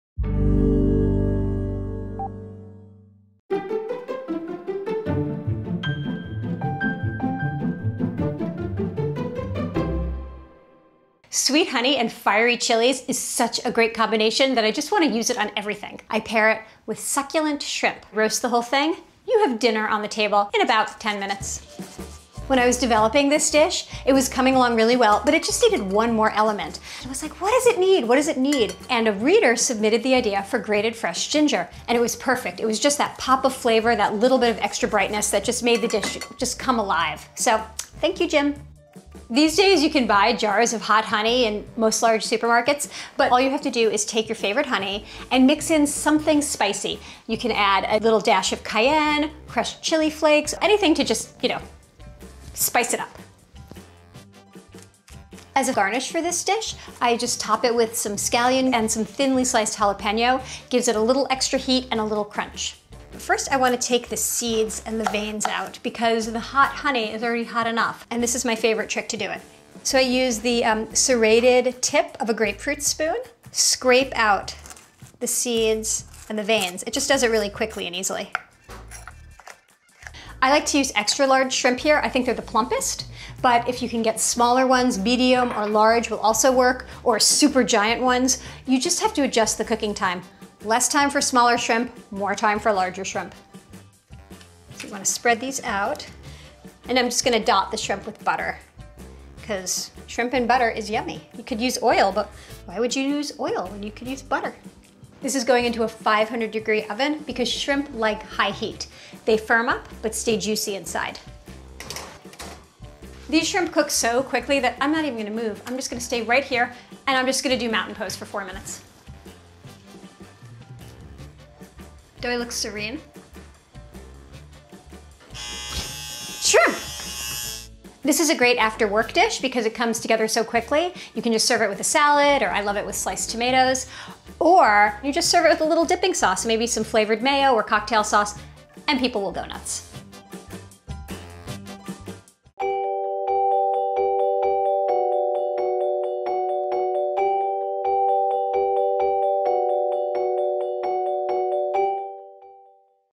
Hot Honey Shrimp | Melissa Clark Recipes | The New York Times
Sweet honey and fiery cayenne create an irritable combo for a quick weeknight shrimp dinner.

Produced by: ALEC COHEN